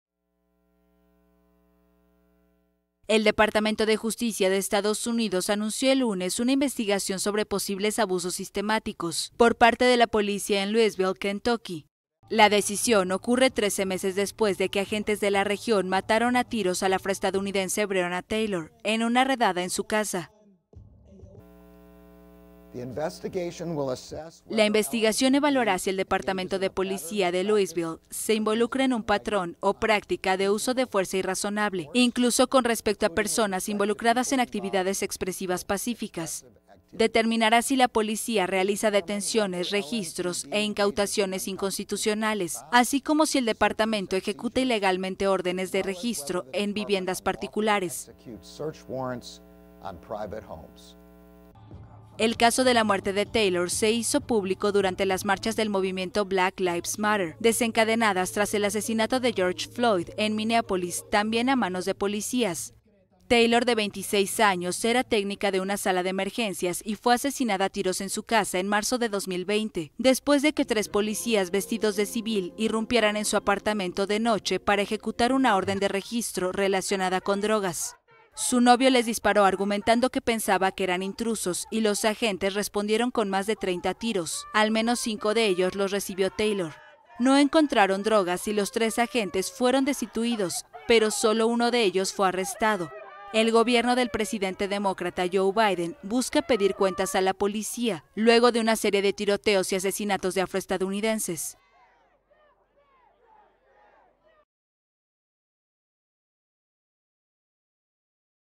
Departamento de Justicia de EEUU investiga a la policía de Louisville por abusos | AFP
El Departamento de Justicia de Estados Unidos anunció el lunes una investigación sobre posibles abusos sistemáticos por parte de la policía en Louisville, Kentucky, 13 meses después de que agentes mataran a tiros a la afroamericana Breonna Taylor en una redada en su casa.